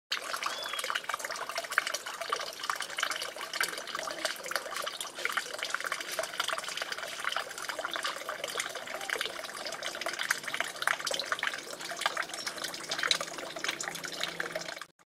OVNIS ESFERAS 30/11/2015
Las esferas son tres y se pierden hacia el este muy rapido, muy silencioso muy alto y un solbrillante que resplandece su metales amigos en HD..